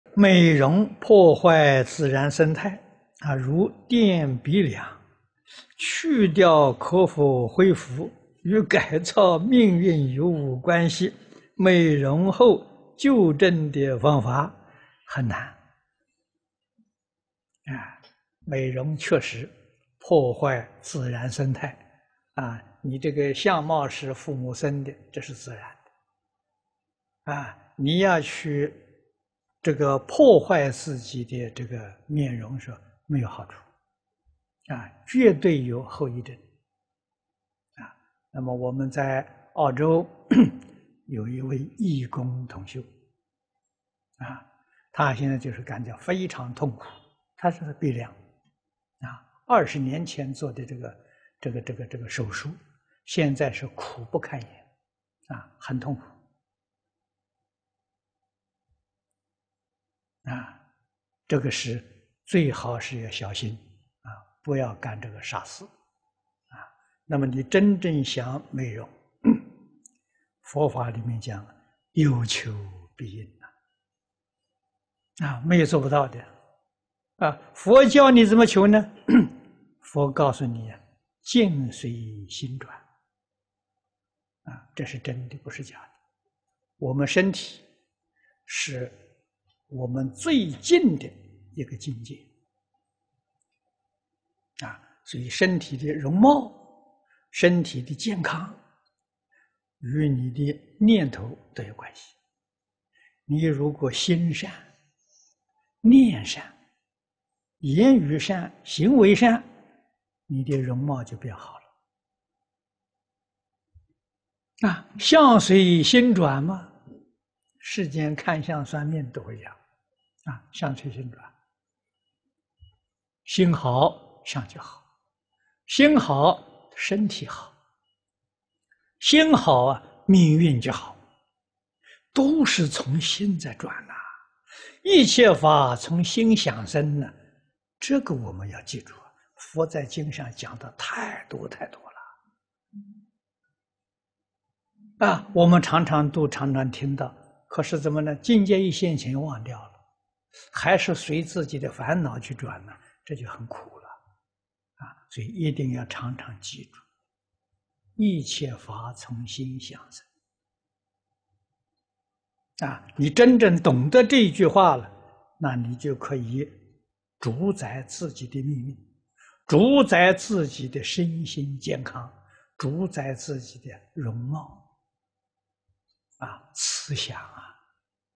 淨空老法師｜學佛答問｜整容後如何恢復？-第1294集
🌿問：美容破壞自然生態，如墊鼻樑，去掉可否恢復？與改造命運有無關係？美容後糾正的方法。

🌿答：很難！美容確實破壞自然生態，你的相貌是父母生的，這是自然的，你要去破壞自己的面容沒有好處，絕對有後遺症。我們在澳洲有位義工同修，她現在就感覺到非常痛苦，她就是鼻樑。二十年前做的手術，現在是苦不堪言，很痛苦，這個最好是要小心，不要幹這個傻事。你真正想美容，佛法裡面講有求必應，沒有做不到的。佛教你怎麼求？佛告訴你「境隨心轉」，這是真的，不是假的。

我們身體是我們最近的一個境界，所以身體的容貌，身體的健康，與你的念頭都有關係。你如果心善、念善、言語善、行為善，你的容貌就變好，相隨心轉！世間看相算命的都會講，相隨心轉，心好相就好，心好身體好，心好命運就好，都是從心在轉。「一切法從心想生」，這個我們要記住，佛在經上講得太多太多了。我們常常讀，常常聽到，可是怎麼？境界一現前忘掉了，還是隨自己的煩惱去轉，這就很苦。所以一定要常常記住，「一切法從心想生」。你真正懂得這句話，你就可以主宰自己的命運，主宰自己的身心健康，主宰自己的容貌，慈祥！

2005/10/7